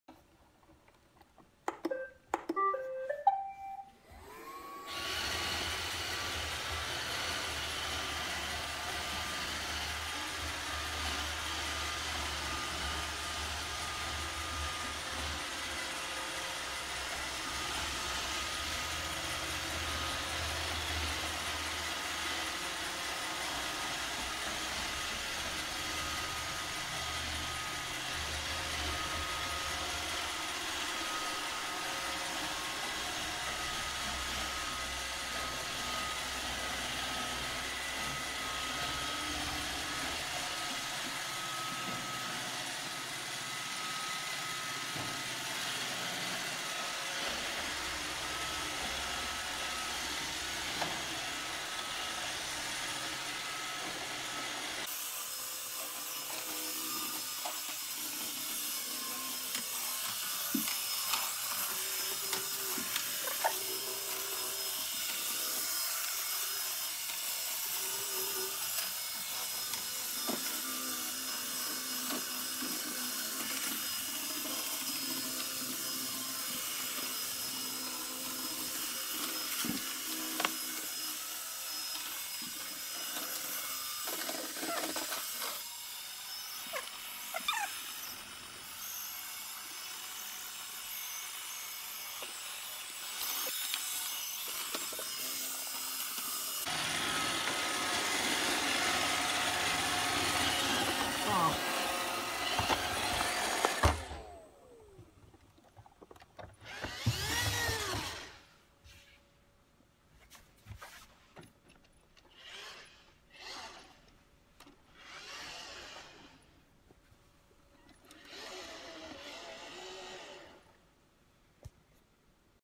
Google Cartographer on cleaning Roomba
This roomba is moving in normal cleaning mode, and the Google Cartographer is makeing a map of the room.